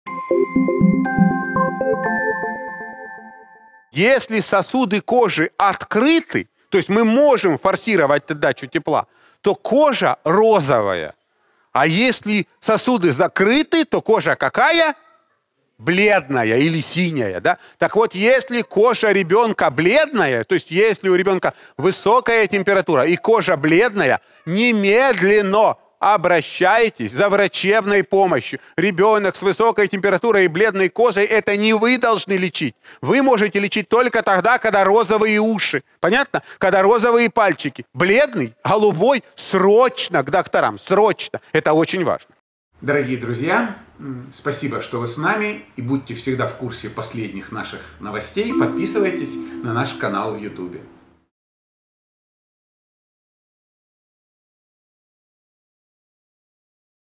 Что нужно делать, если при высокой температуре ребёнок бледный? - Доктор Комаровский
Доктор Комаровский обратит внимание родителей на то, что если ребенок при высокой температуре бледный, значит, срочно нужен врач.

Doctor Komarovskiy - What should you do if a child at high temperature pale?